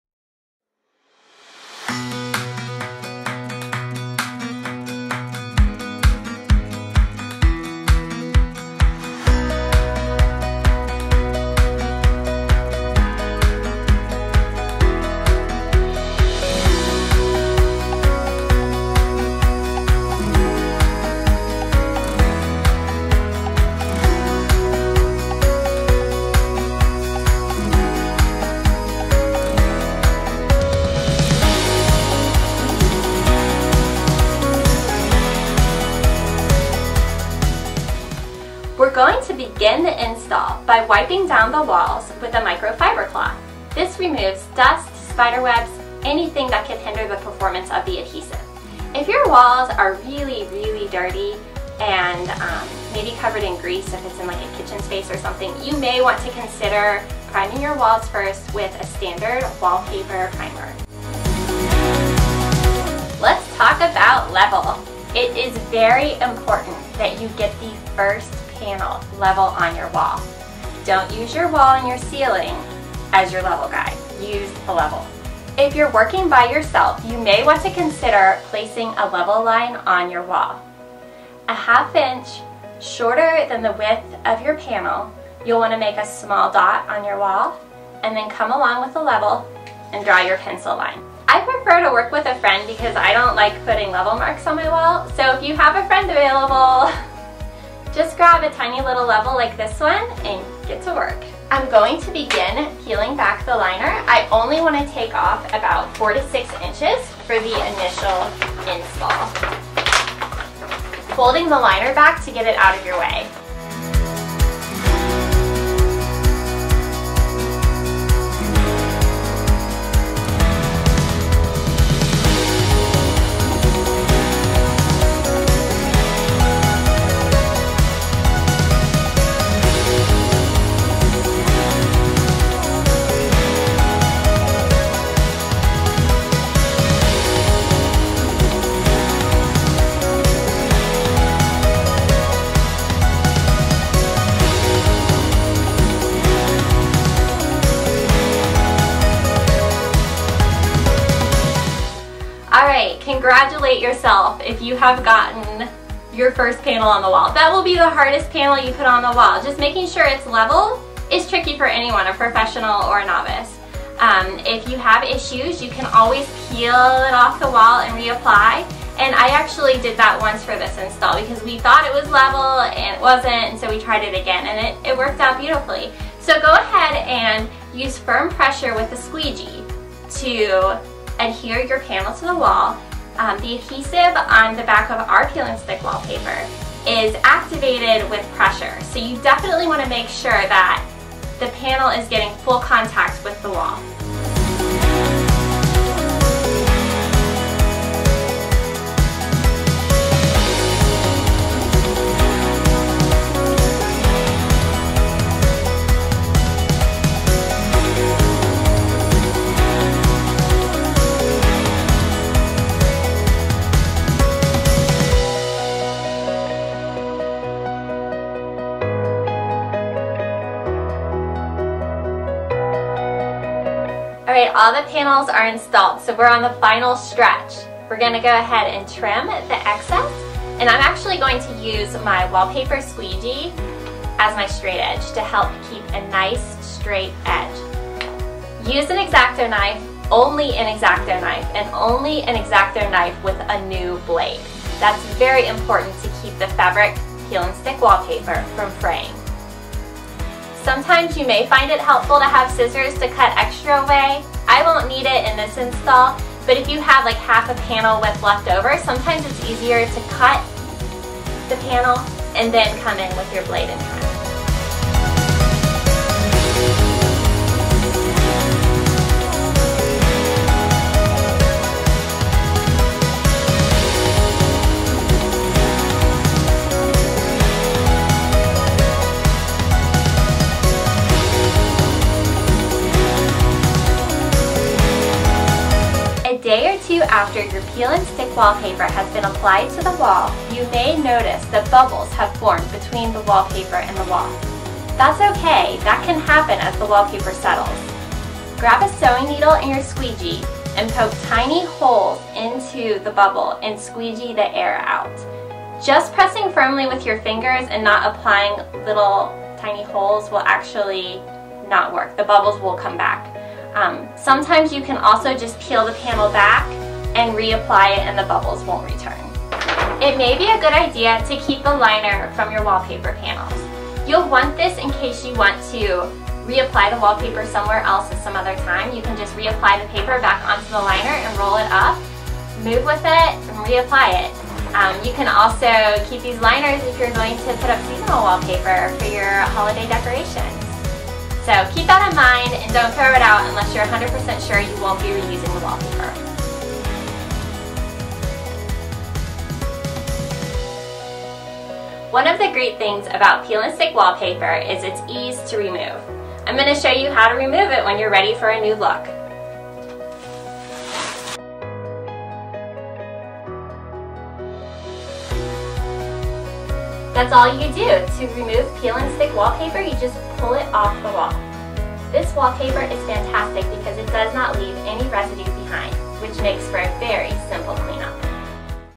Wallaby Walls - Peel and Stick Installation
How to install Wallaby Walls peel and stick wallpaper.